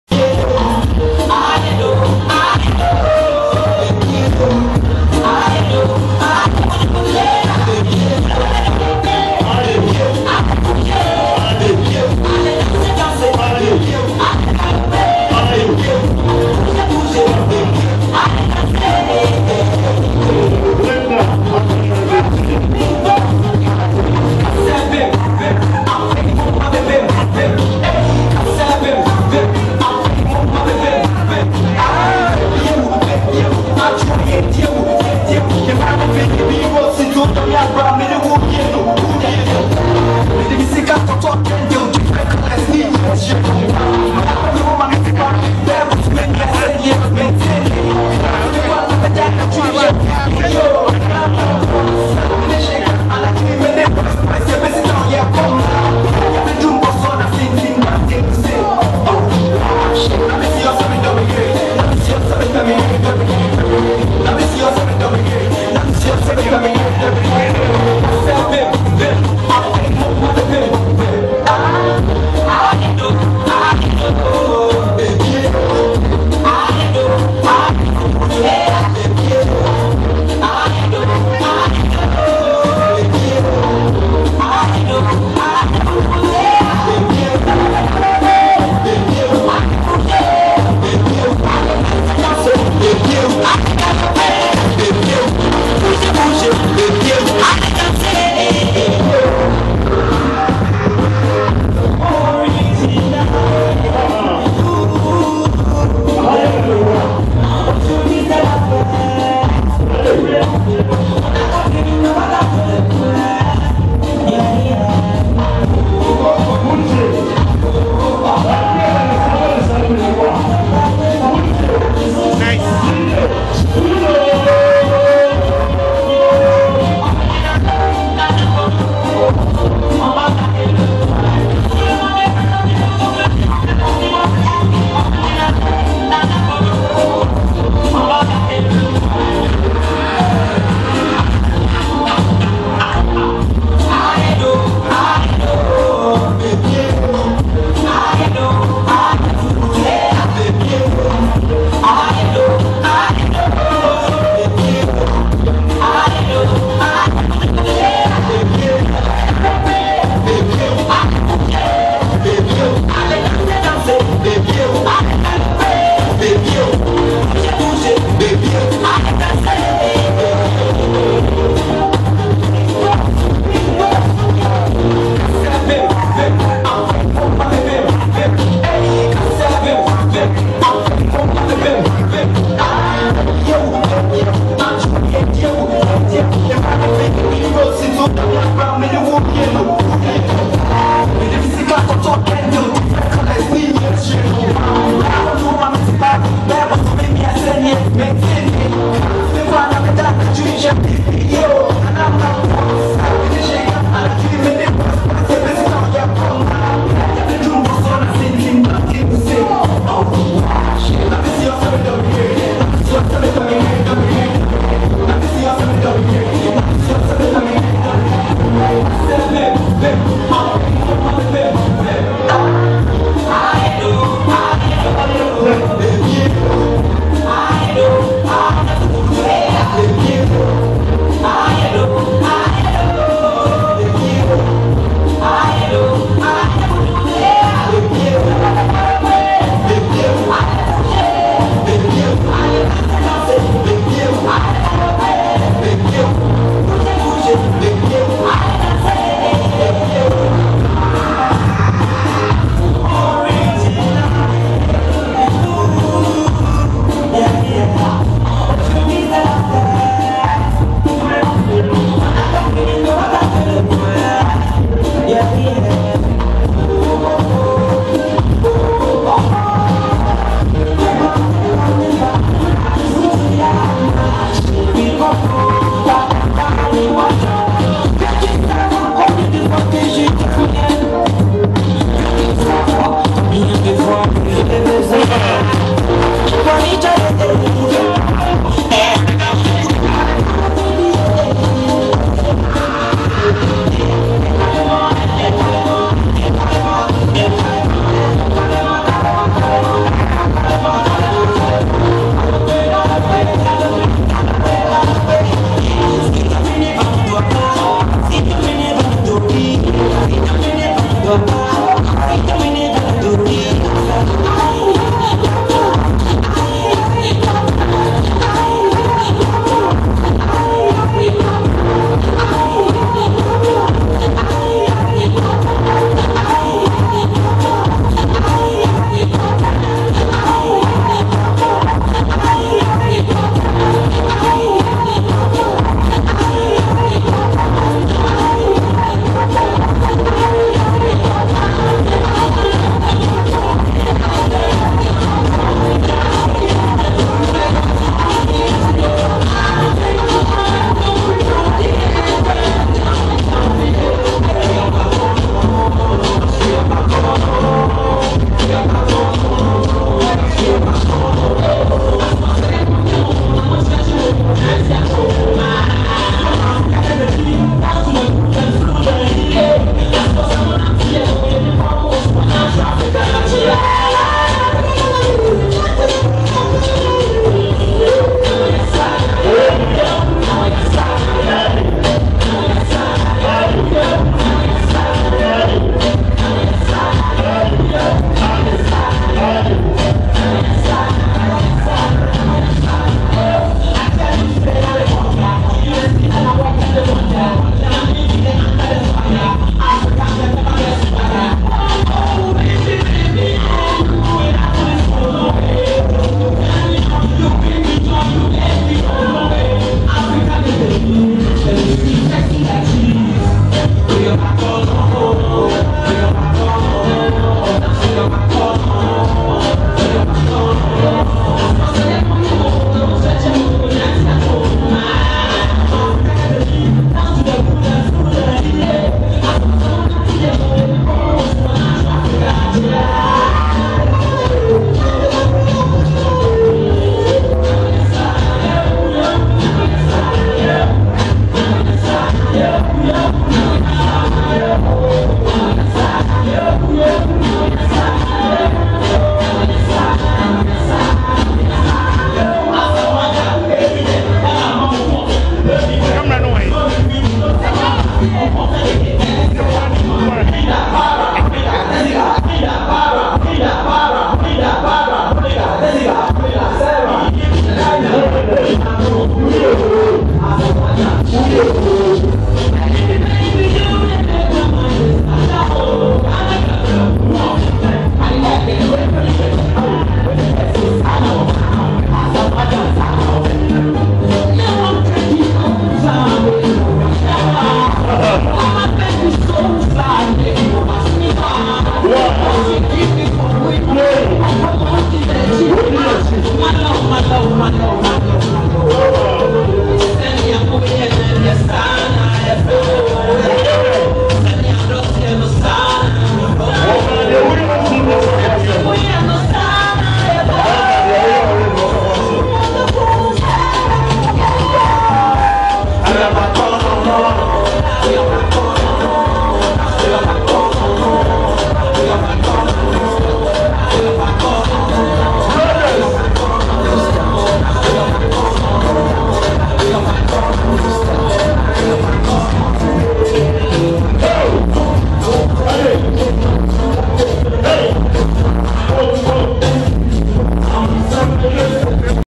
Ghana In Toronto - kokorokoo - Ebenezer's 50th Birthday
May 15, 2011 - Amansie multicultural center - Great night of fun. happy birthday Eben.